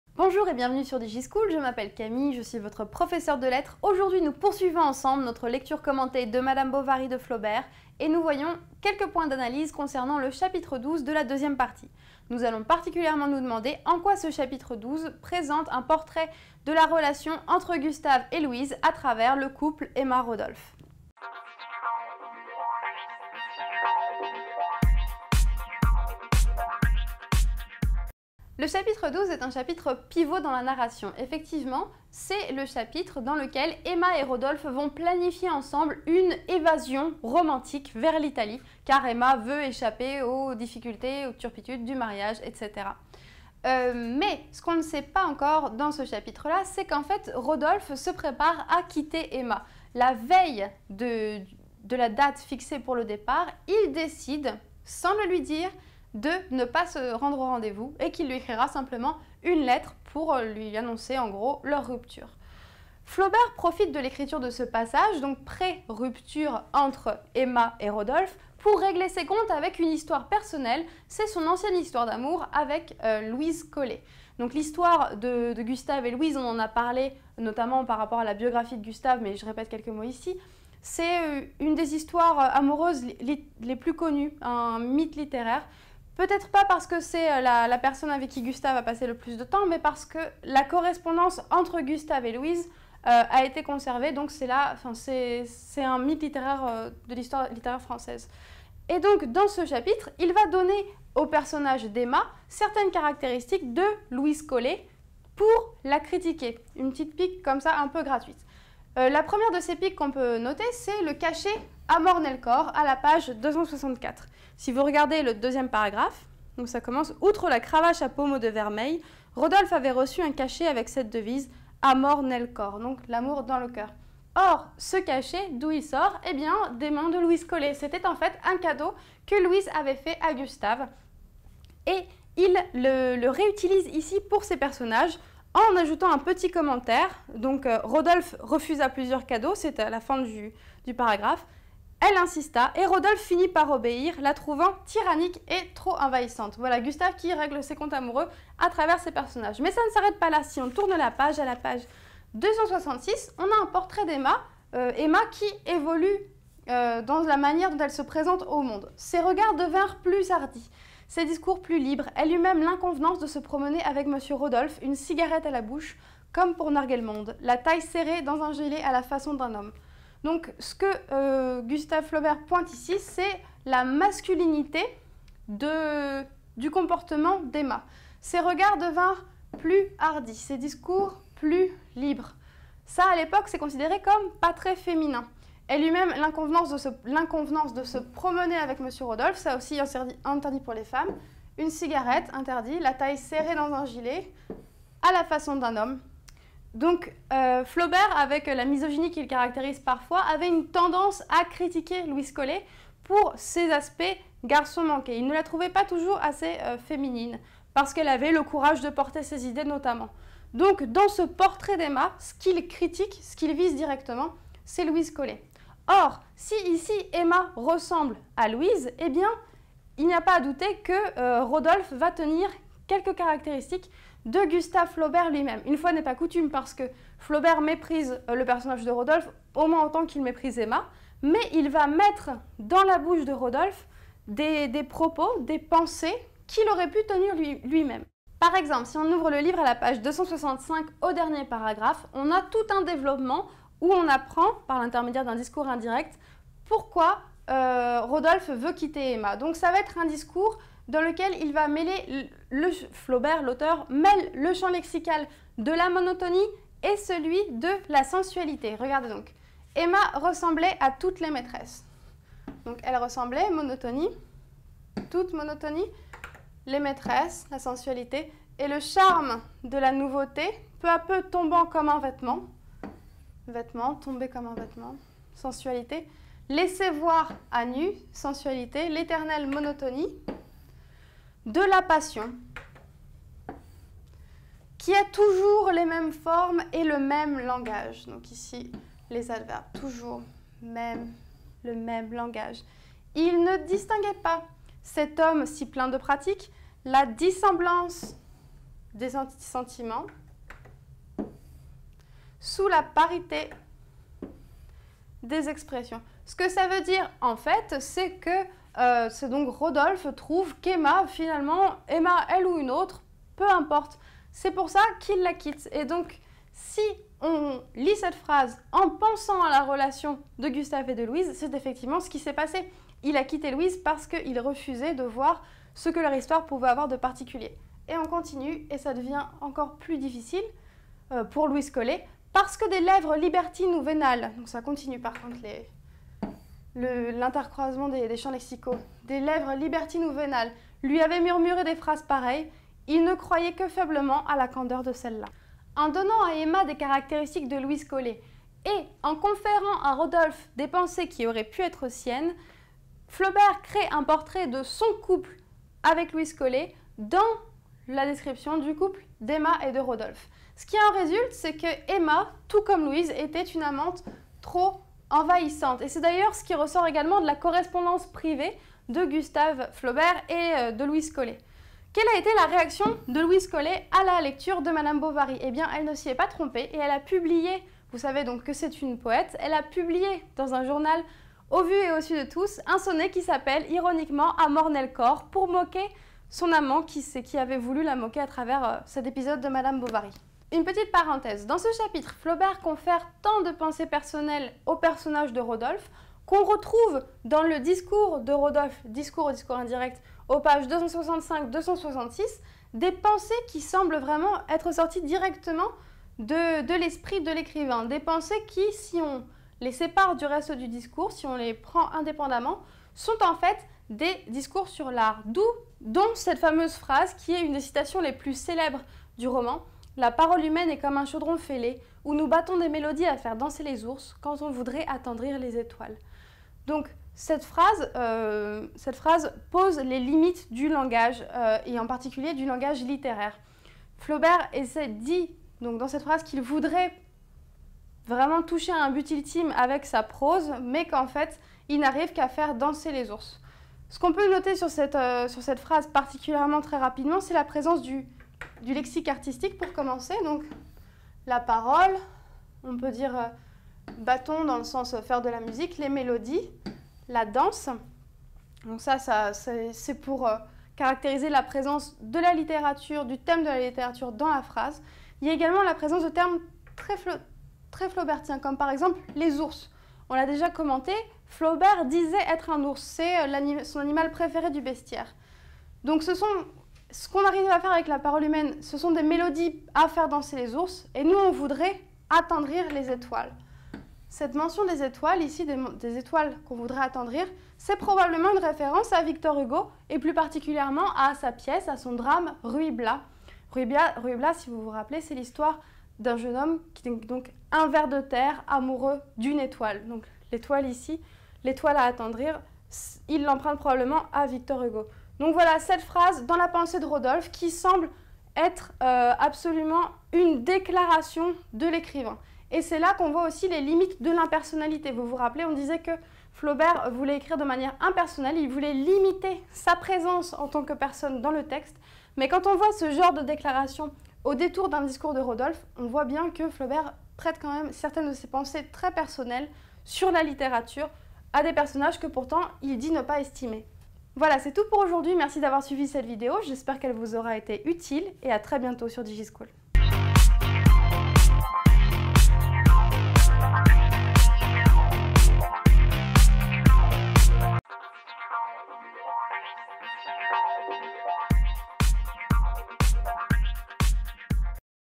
Lecture commentée de Madame Bovary - partie II - chapitre 12 - points d’analyse 1 - Littérature
pour toujours plus d'infos sur le bac L !

Dans cette vidéo, notre professeur de littérature vous présente des points d'analyse du chapitre 12 de la seconde partie de Madame Bovary.

Il s'agit d'un chapitre pivot dans la narration. Les deux amants Emma et Rodolphe planifient une fuite vers l'Italie. Or, la veille du départ, Rodolphe décide d'abandonner Emma.

Pour expliquer le choix de Rodolphe, Flaubert mêle le champs lexical de la monotonie et celui de la sensualité. Flaubert crée un portrait du couple de Rodolphe et Emma, à l'image celui de son couple avec Louise Collet.